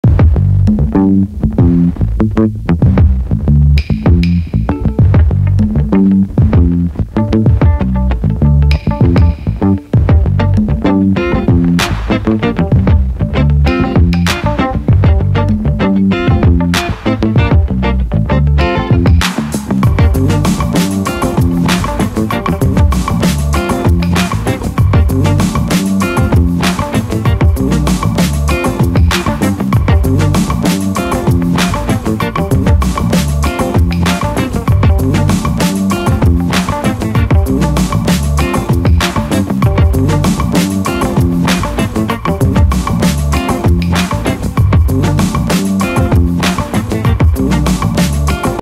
Any doubts about charging mobile you must watch
In this video I'm giving 6 statements that is better to know about charging mobile,why mobile going to heat,speed of ram ,background apps and which type of charger we should use for our mobile